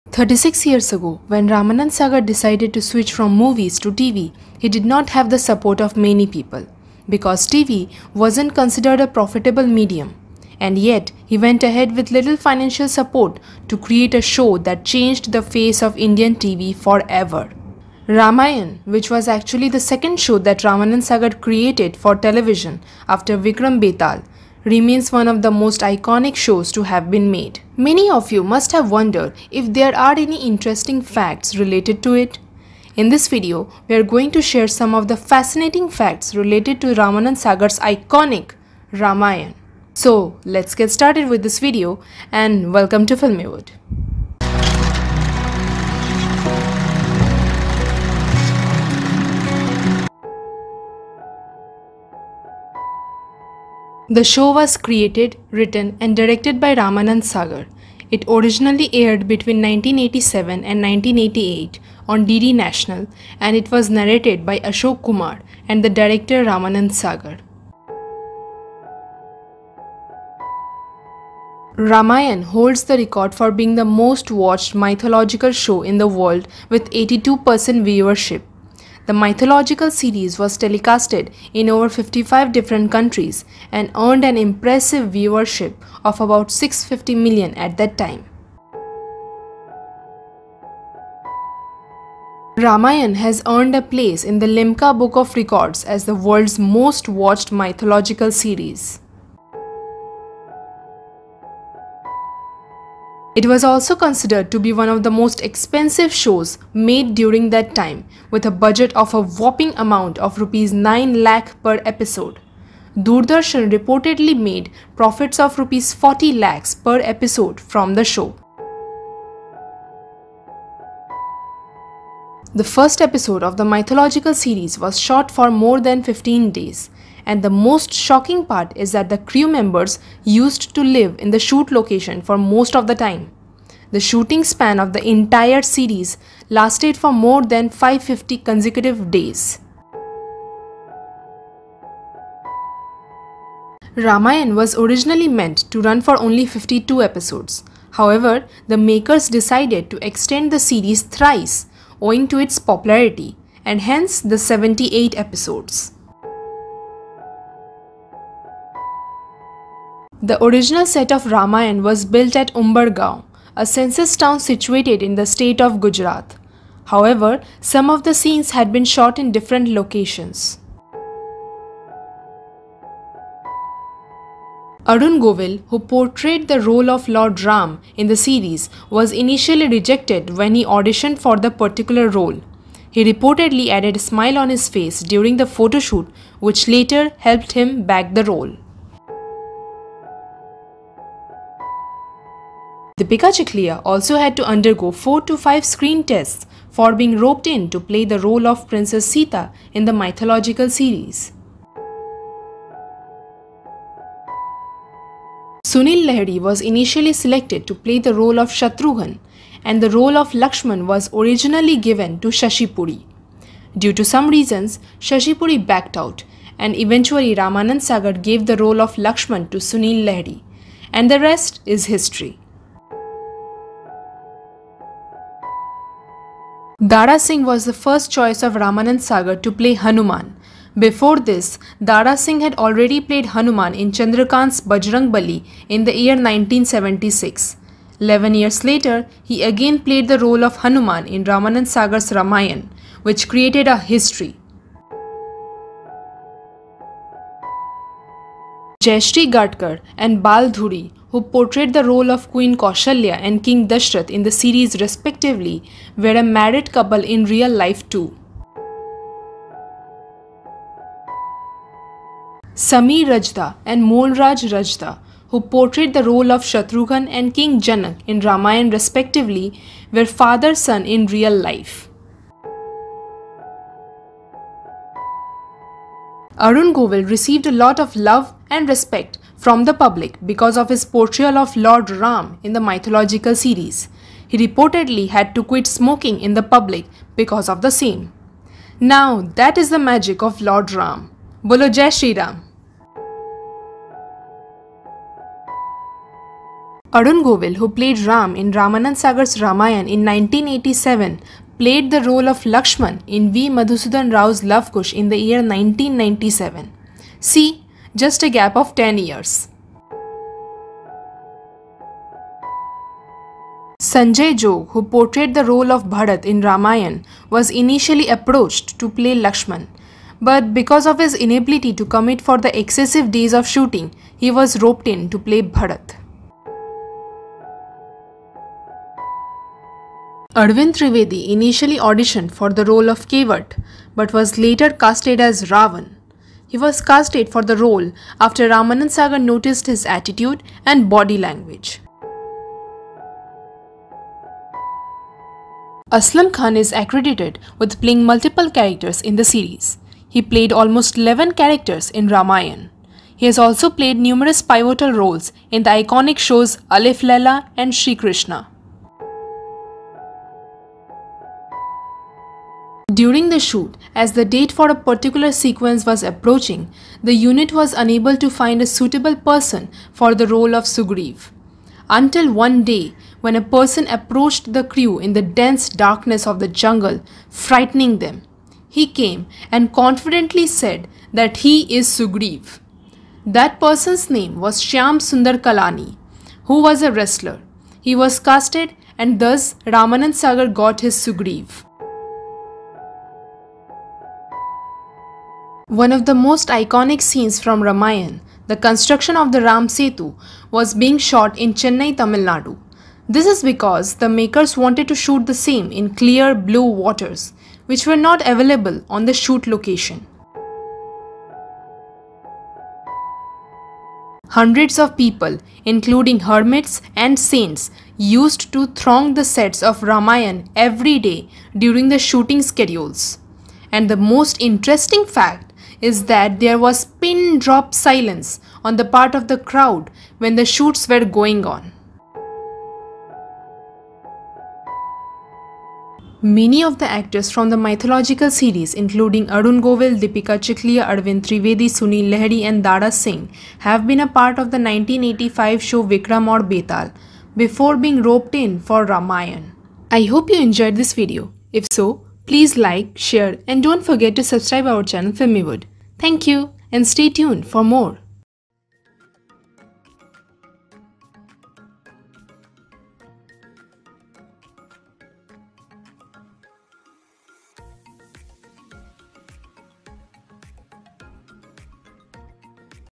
Ramanand Sagar's RAMAYAN | Amazing Facts | Arun G, Deepika C, Sunil L, Arvind T, Dara S | Filmywud
Hello Everyone..!!🙋‍♀️

Ramanand Sagar's RAMAYAN | Amazing Facts | Arun Govil, Deepika Chikhlia, Sunil Lehri, Arvind Trivedi, Dara Singh | Filmywud

In this video, we are going to share some interesting facts about Ramanand Sagar's Iconic RAMAYAN.

The show was created, written, and directed by Ramanand Sagar. It originally aired between 1987 and 1988 on DD National.

Ramayan holds the record for being the most watched mythological show in the world.

NO COPYRIGHT HAS BEEN INTENDED IN THIS VIDEO.

Regards & Thank You..!!